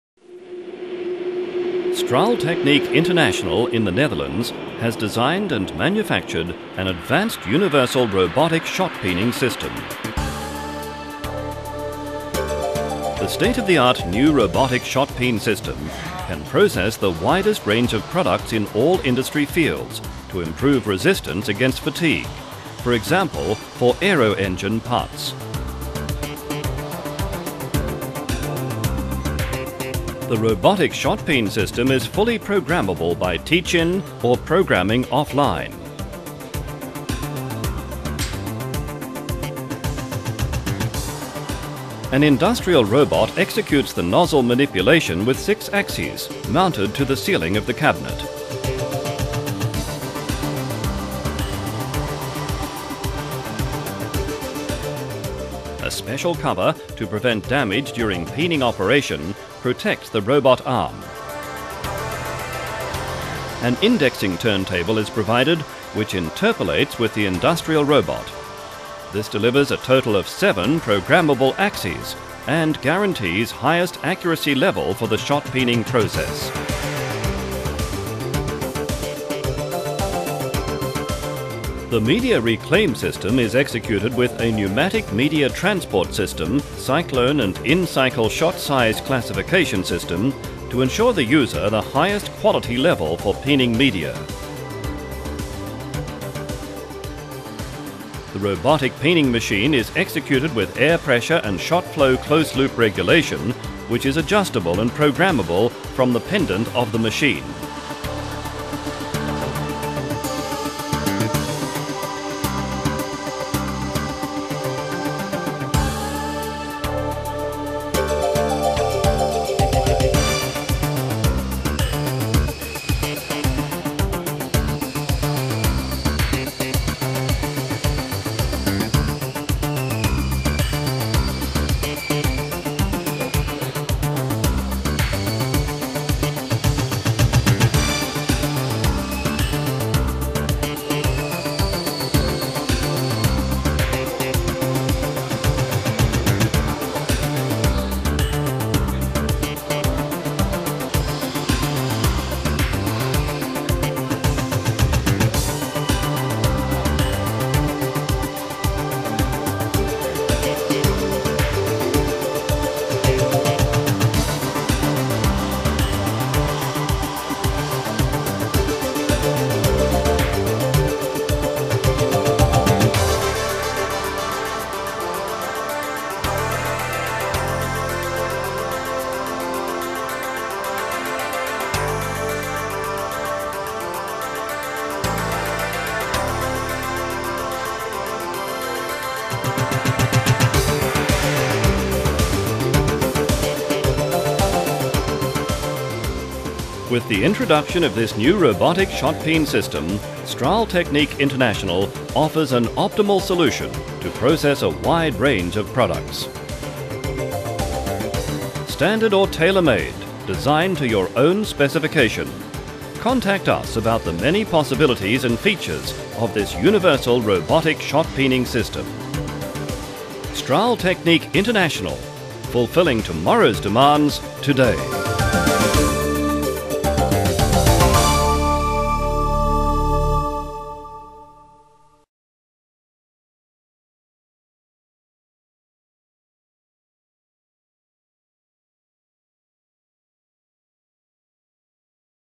Robotic Shotpeening Machine
Straaltechniek's Multi-purpose Robotic shotpeening machine. Multi-purpose shotpeening machine to induce compressive residual stresses in the top layer of products, exposed to fluctuating loads. Main purpose of this treatment is extension of life time for products.